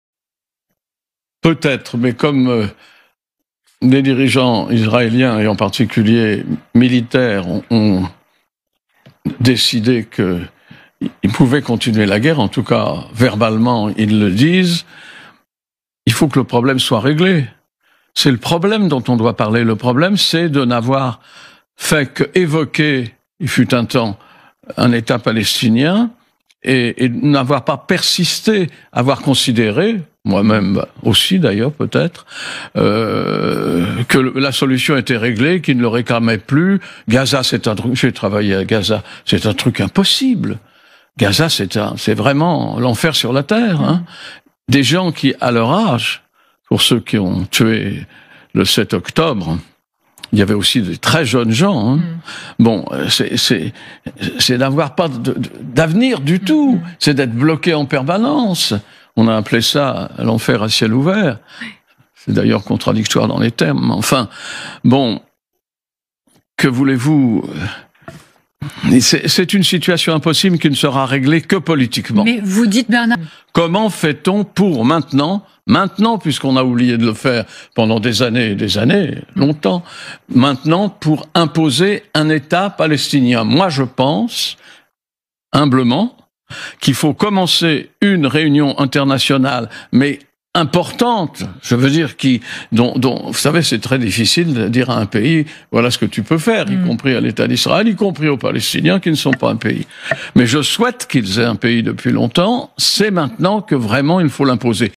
Kouchner:"J'ai travaillé à Gaza, c'est un truc impossible, Gaza,c'est vraiment l'enfer sur la terre"
Bernard Kouchner: "J'ai travaillé à Gaza, c'est un truc impossible, Gaza, c'est vraiment l'enfer sur la terre"